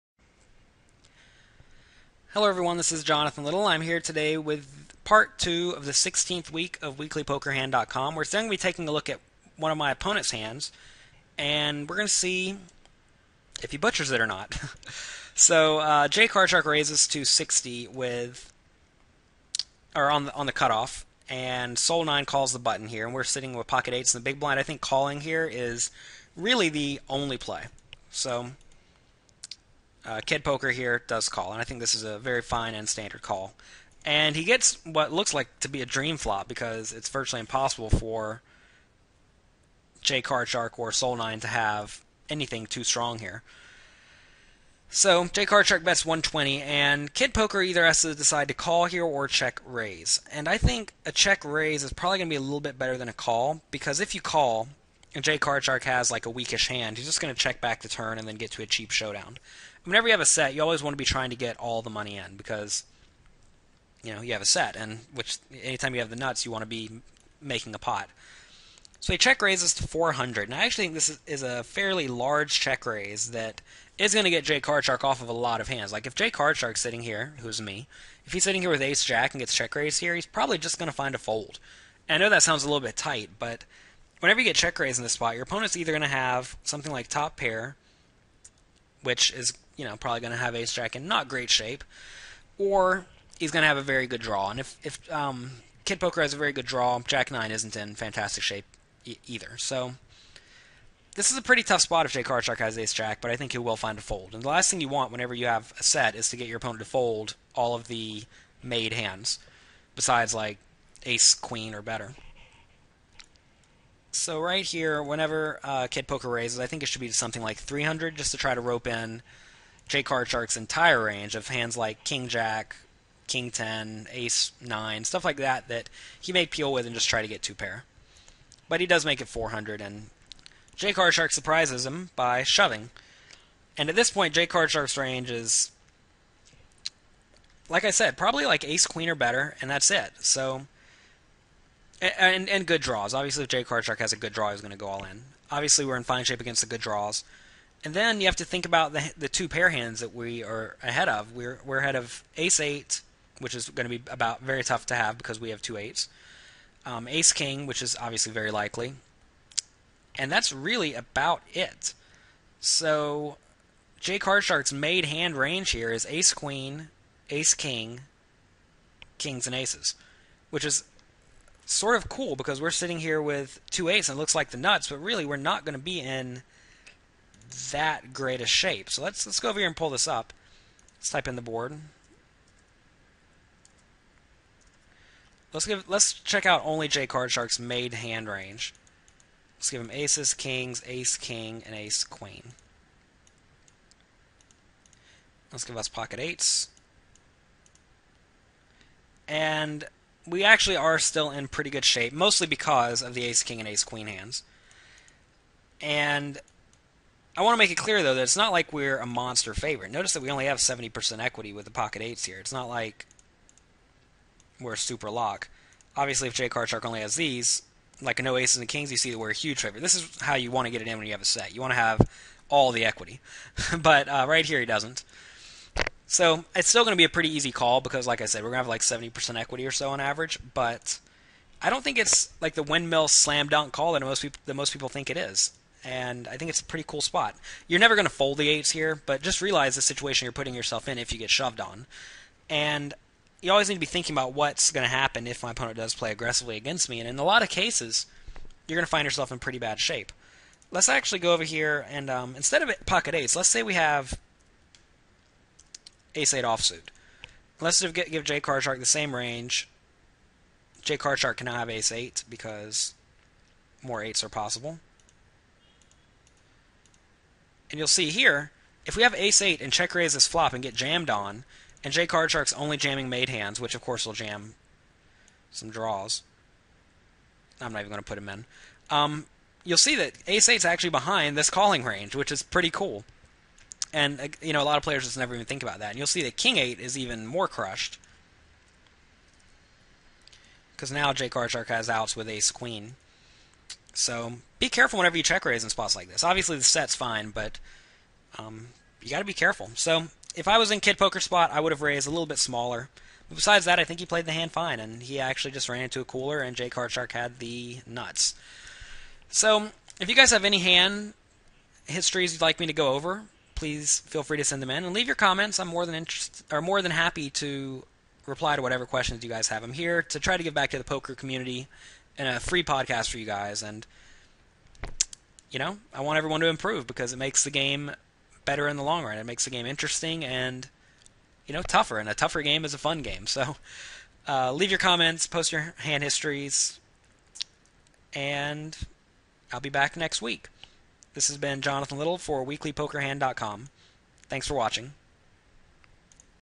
WeeklyPokerHand.com - Week 16 Part 2 of 2
Weekly poker hand analysis podcast from Jonathan Little - Week 16 Part 2 of 2.  Here's same hand from Part 1, analyzed from my opponent's perspective.